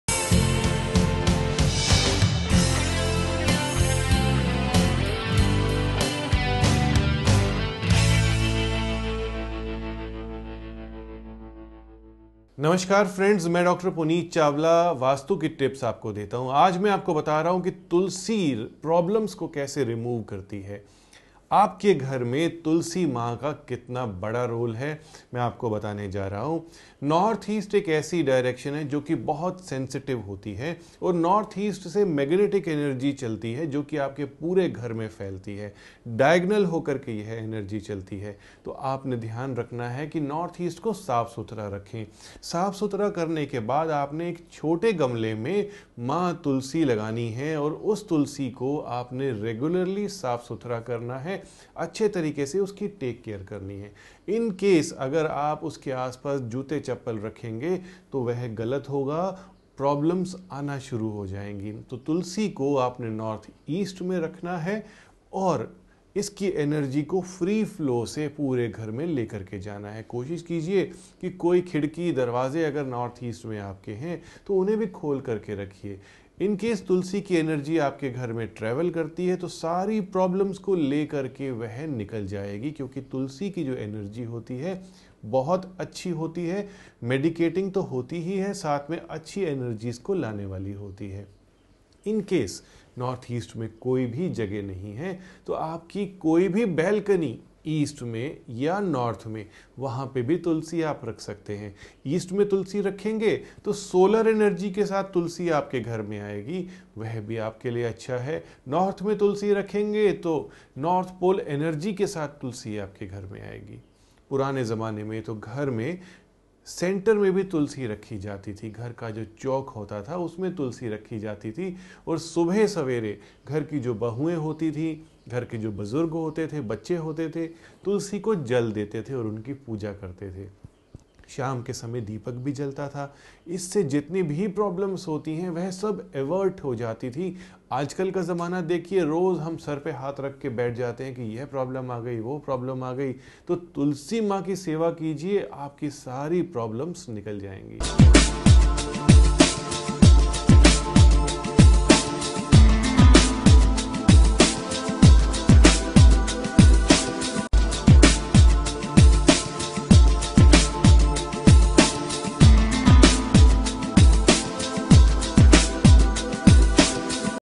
तुलसी की पूजा बनाये बिगड़े हुआ काम | Tulsi Plant Vastu | Dr Puneet Chawla
Vastu Tips: Why do We Worship the Tulsi Plant ?
Dr Puneet Chawla is an Expert Vastu Consultant. He Suggest Remedies without demolition using Scientific Logic only. In this Videos Dr Puneet Chawla Explained how Tulsi Plant removes the problems. Keep the North East corner area clean and apply Tulsi Plant.

Join Our Whatsup Group For New New Daily Vastu Upadtes